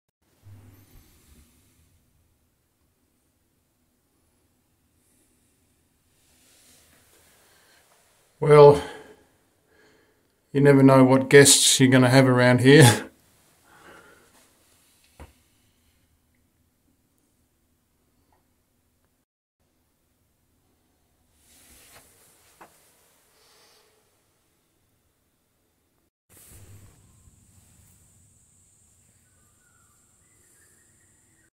Insect/bug house guest, how to get rid of him
A bug insect comes to visit in my house and how to get him out of the house